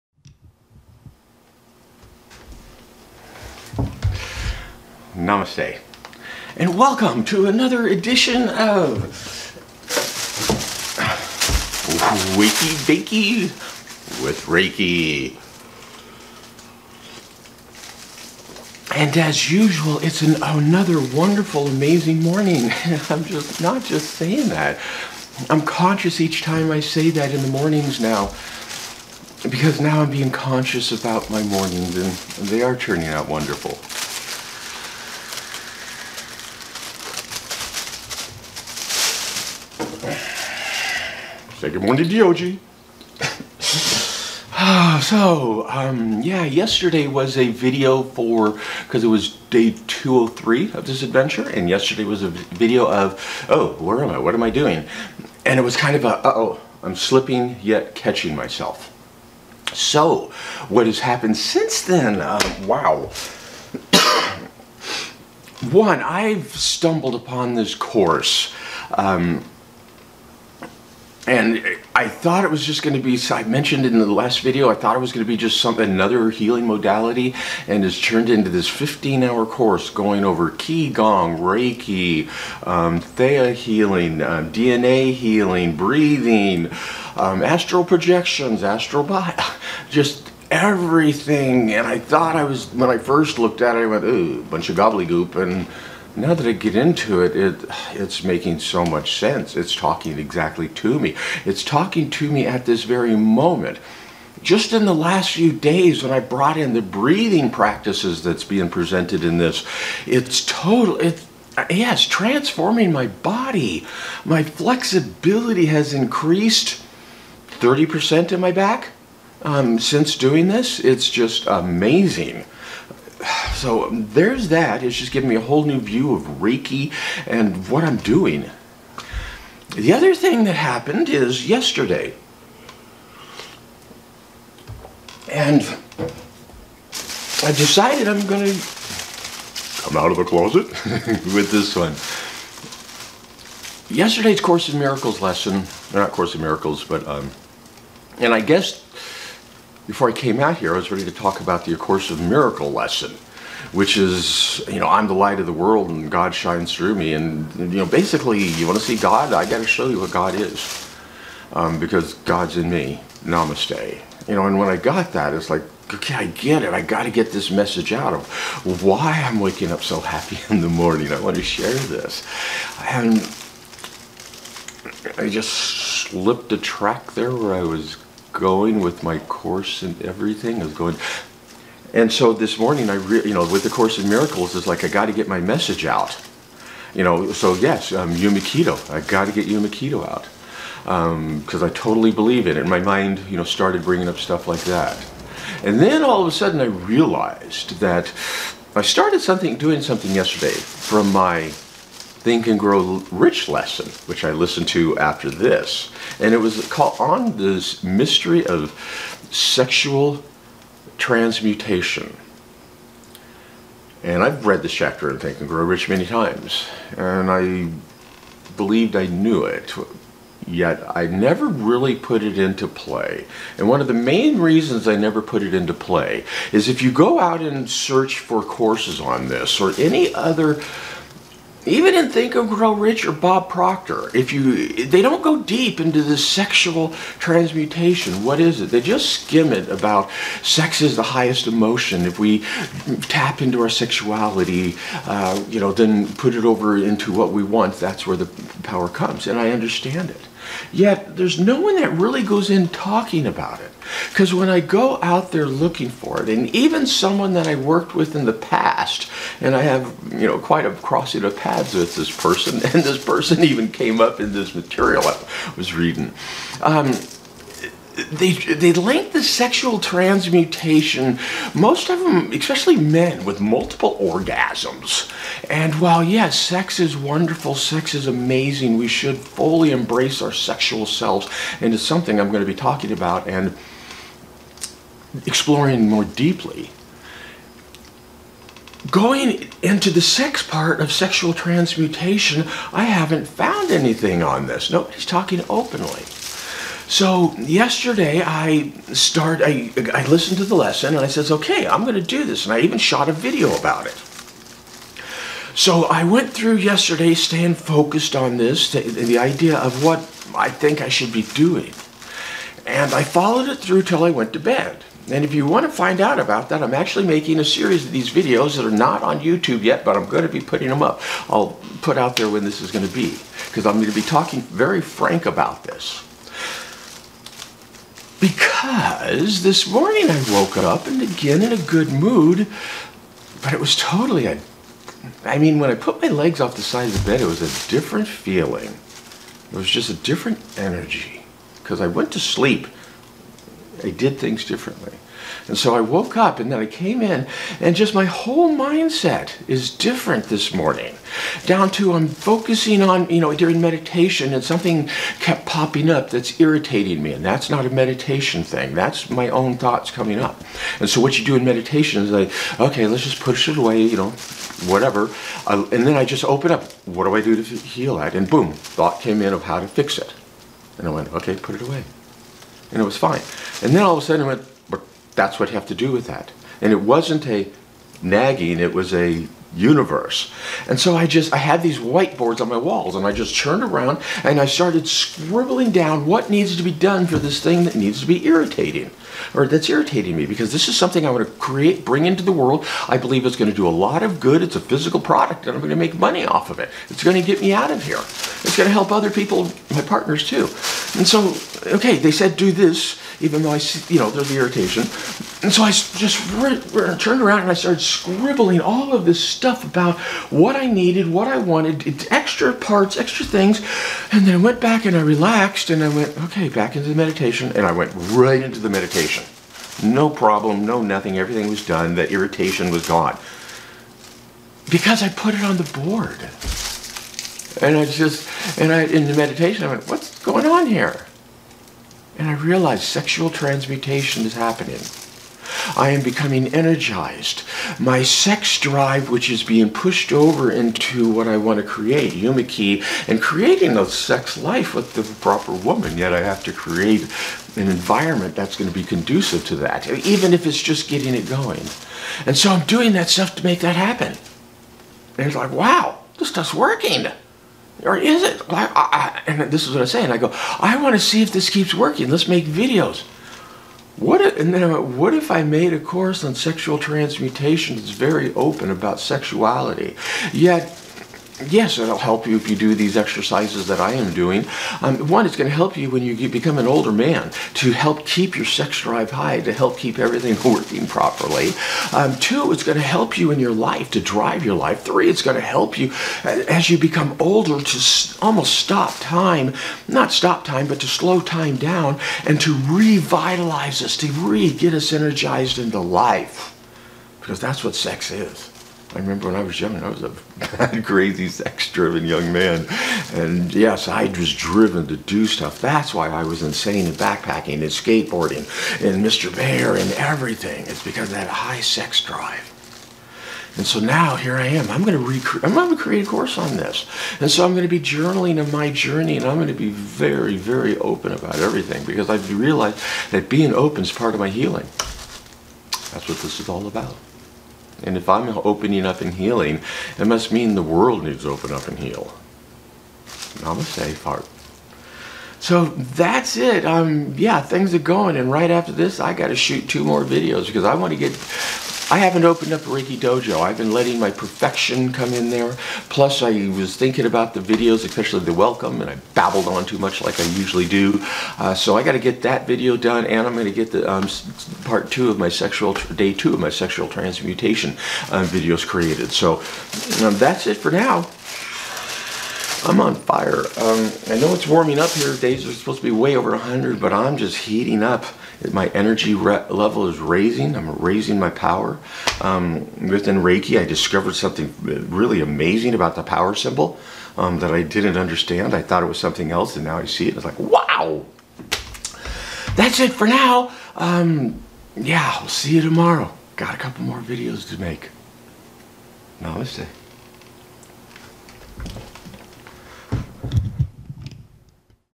Adventures into Sexual Transmutation
I've started an adventure into a deeper understanding of Sexual Transmutation.
Follow me and see where I end up as I make 1 video a day for 1000 days.